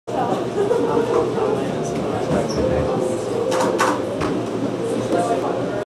UoM Bus Drives By. From Belly Deli Restaurant, Ann Arbor, MI.
2019 March 23, about 6pm.  Looking out the cylindrical lens window at Belly Deli Restaurant, South University Ave, Ann Arbor, MI.  Watching a University of Michigan bus drive by. 1920x1080,  5.763 seconds.